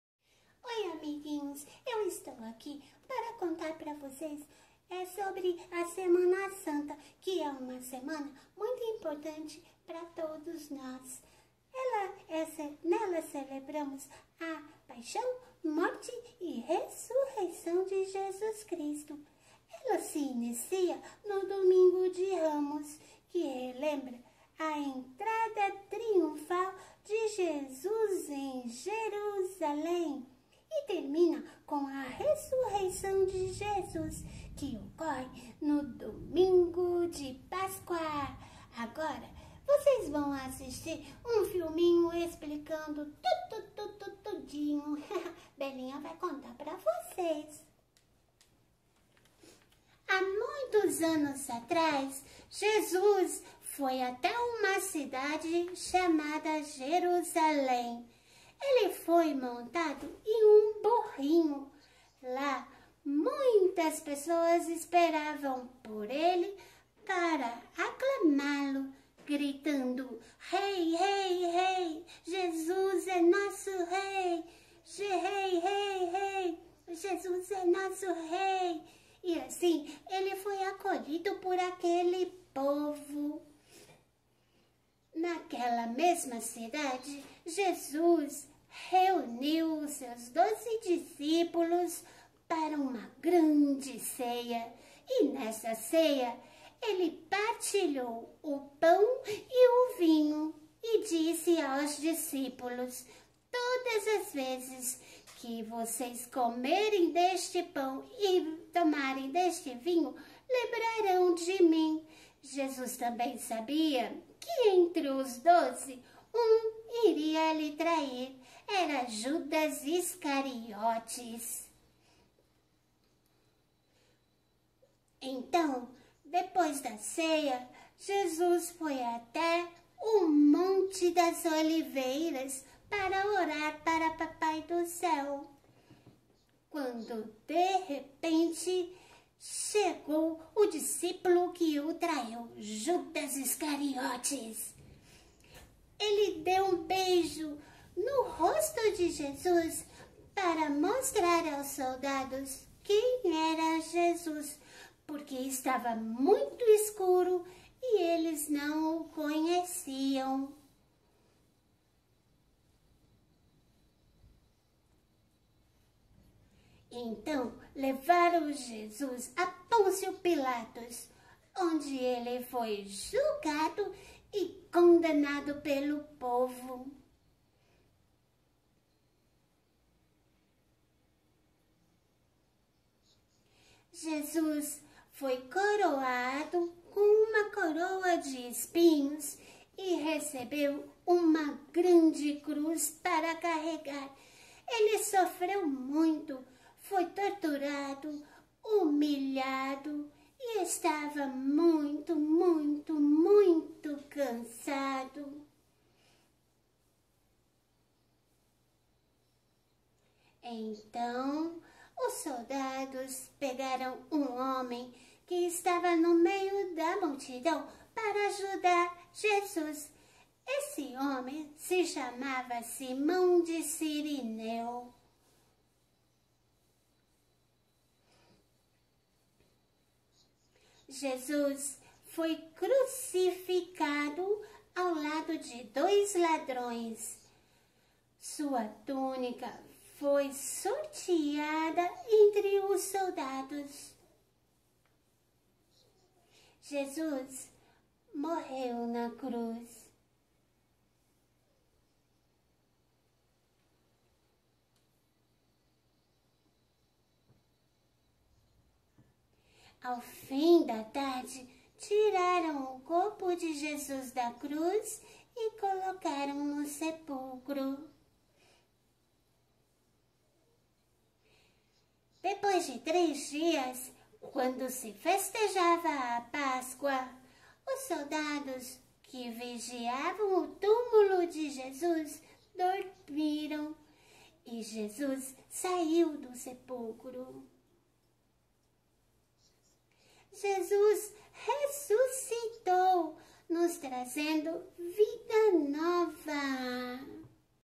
CRIANÇAS, VOCÊS SABEM A IMPORTÂNCIA DA SEMANA SANTA?
Neste domingo iniciamos a Semana Santa 2023. Confira neste vídeo uma explicação bem didática da Belinha para que a criançada entenda a importância e o significado da nossa Semana Maior.